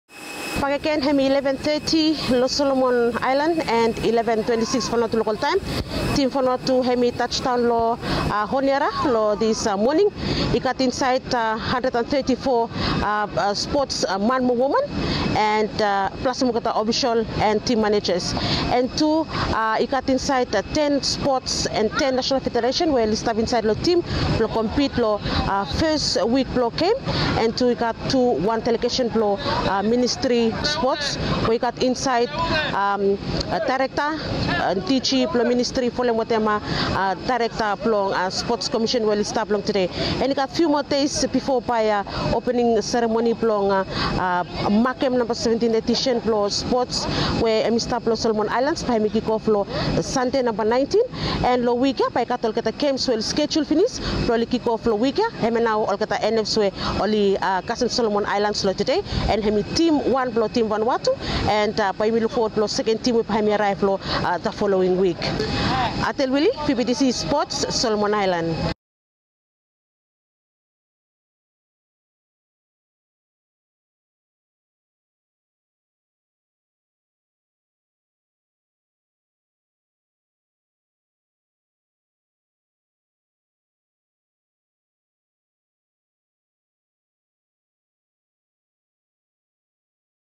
Team Vanuatu arrive in Honiara - SOL2023 Pacific Games | VBTC News
Vanuatu team selected across various disciplines following home ground training for sol2023 Pacific Games.

After a successful arrival in Honiara, Team Vanuatu is gearing up for their initial games at the Pacific Games 2023. The official opening ceremony is scheduled for November 18, 2023, marking the commencement of the highly anticipated sporting event.